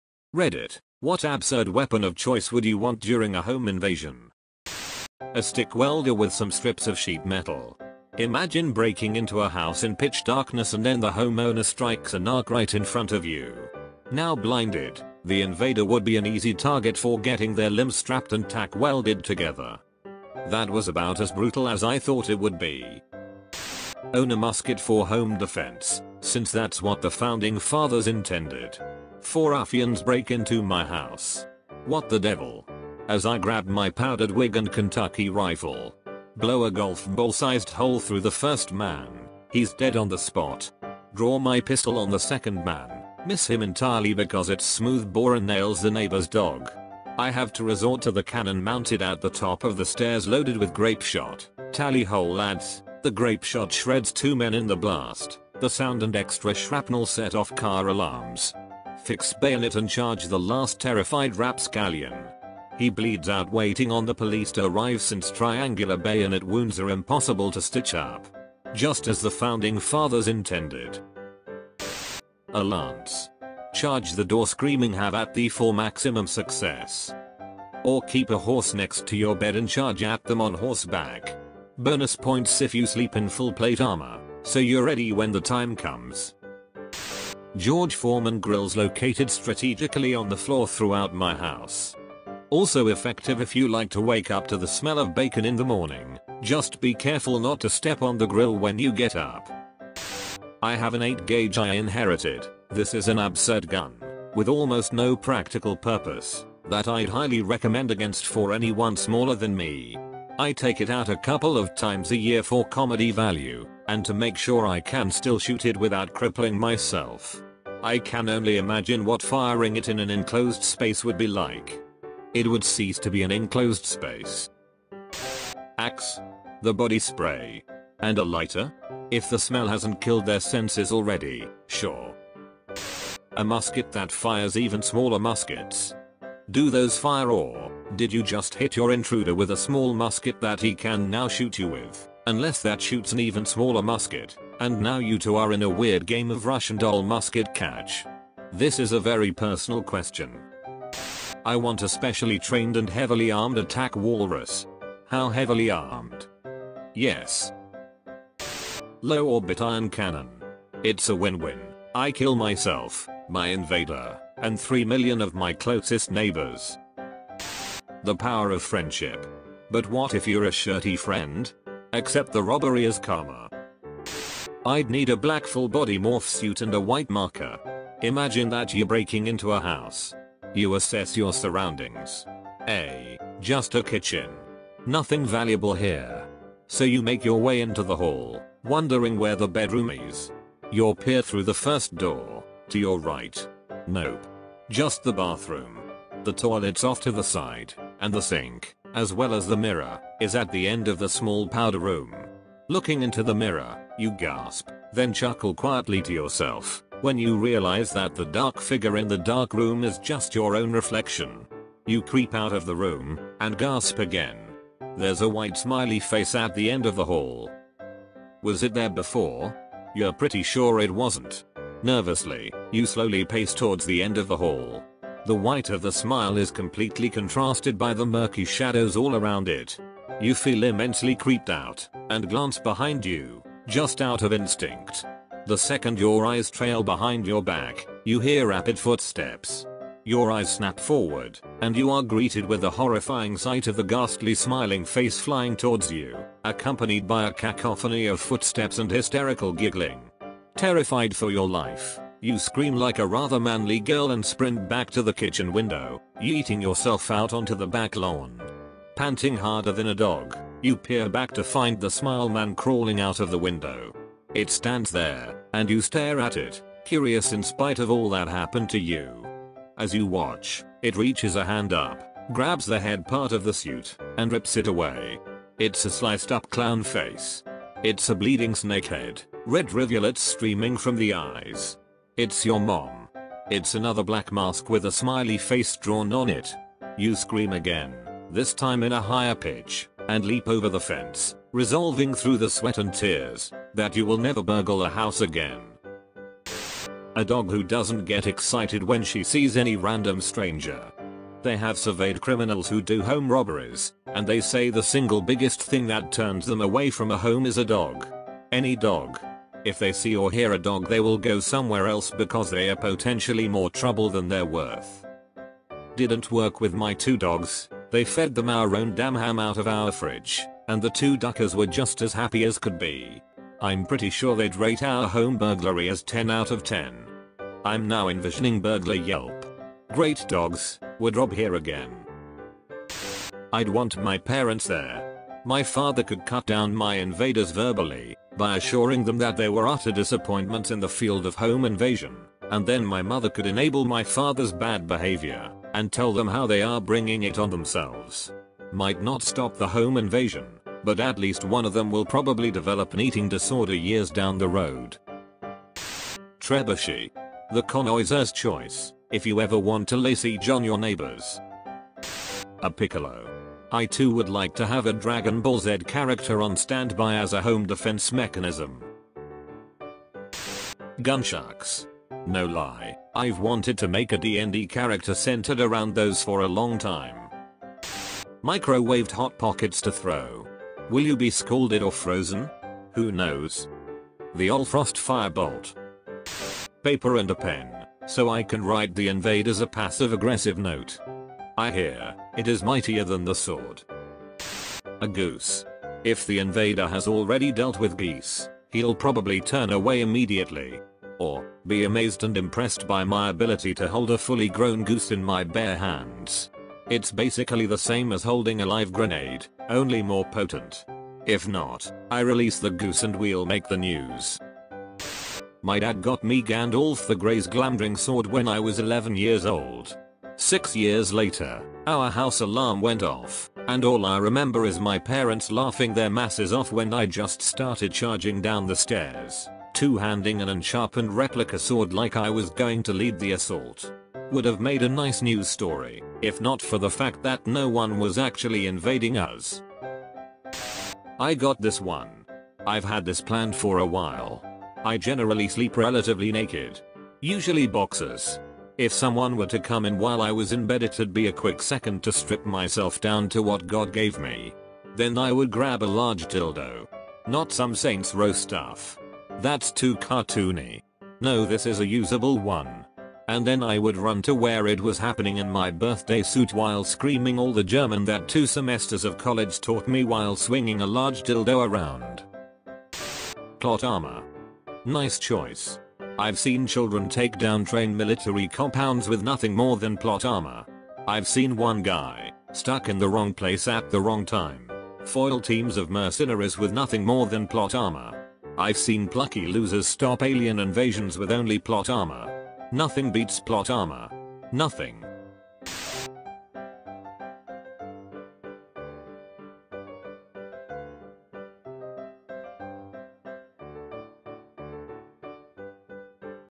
You've Got a Home Invasion Situation, What Absurd Weapon Do You Choose? (r/AskReddit)
r/AskReddit What Ridiculous Weapon Would You Like To Have During a Home Invasion?